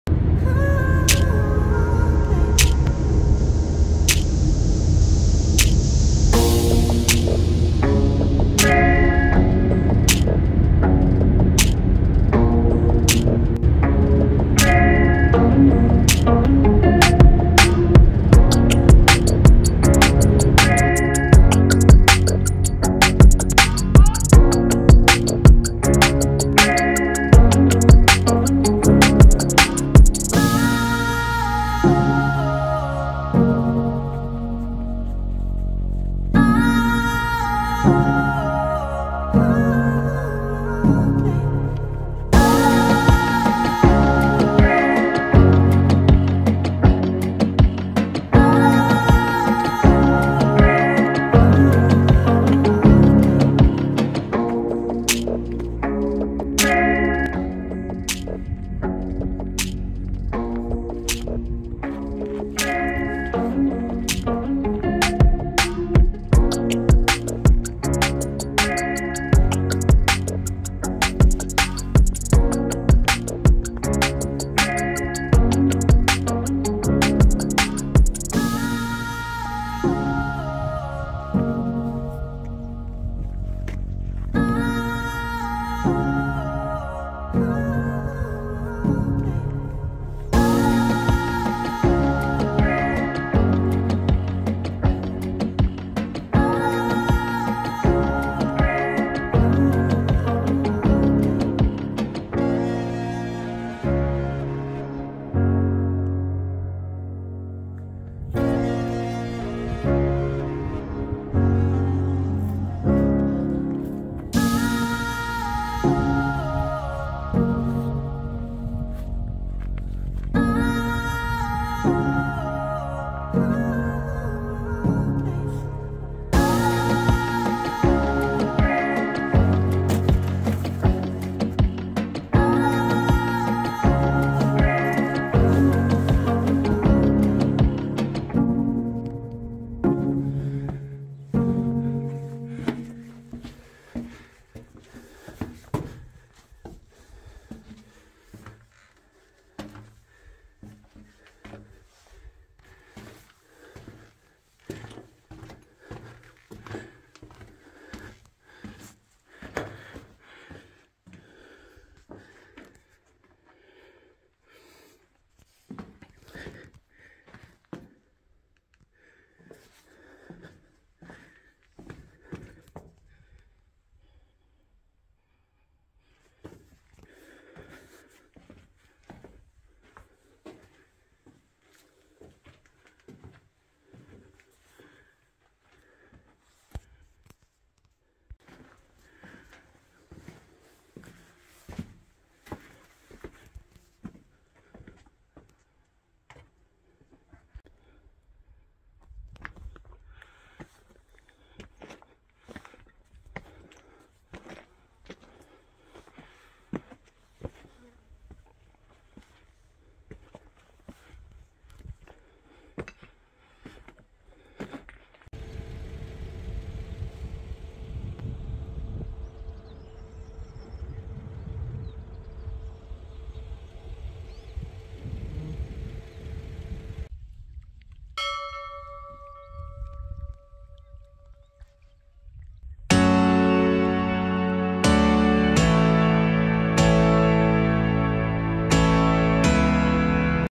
Загадочная пещера и малоизвестное озеро в Азербайджане
Загадочная пещера в Гобустанском районе